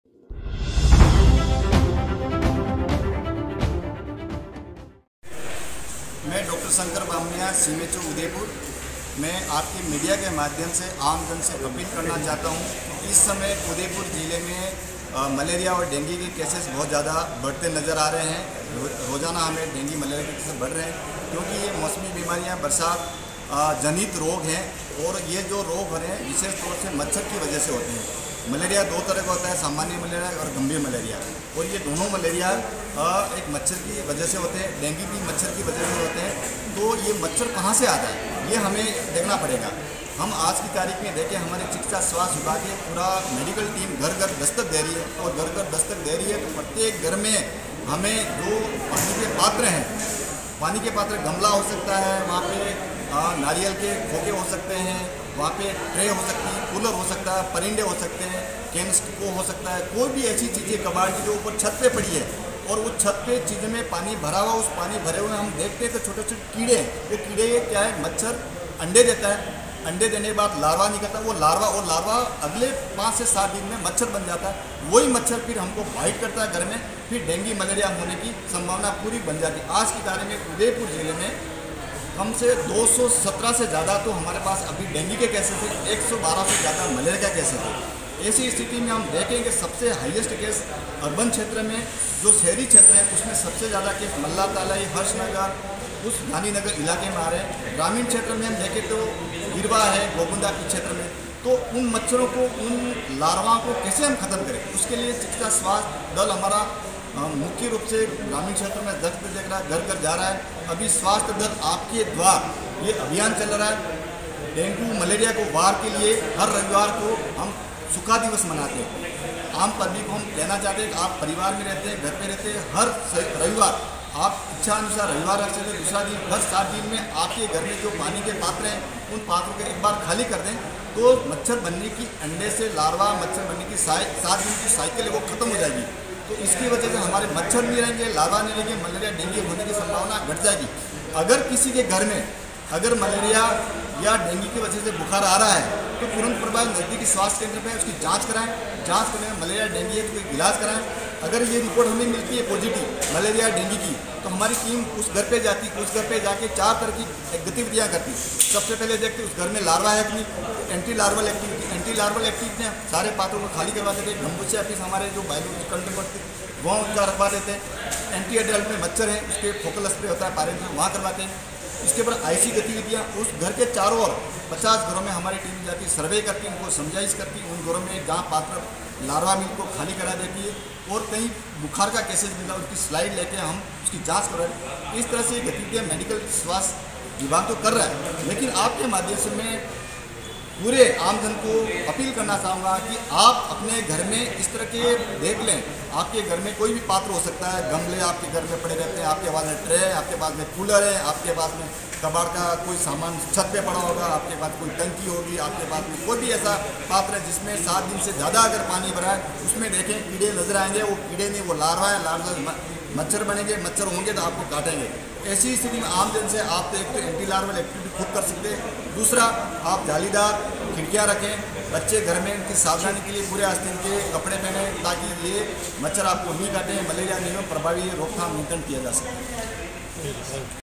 मौसमी बीमारियों विशेषकर डेंगू , मलेरिया के बारे में आमजन को जागरूक करने हेतु प्रेस वार्ता
सीएमएचओ डॉ शंकर बामनिया की प्रेस वार्ता

सूचना केंद्र में आयोजित प्रेस वार्ता को संबोधित  डॉ बामनिया

हर रविवार डेंगू पर वार अभियान जरिए मच्छरों का खात्मा करने का आह्वान

शहर के 12 सेक्टरों में 30 से ज्यादा टीमें कर रही है काम : डॉ बामनिया

24 नर्सिंग महाविद्यालयों का लिया जा रहा है अभियान में सहयोग : डॉ बामनिया

डिप्टी सीएमएचओ डॉ अंकित जैन भी है प्रेस वार्ता में मौजूद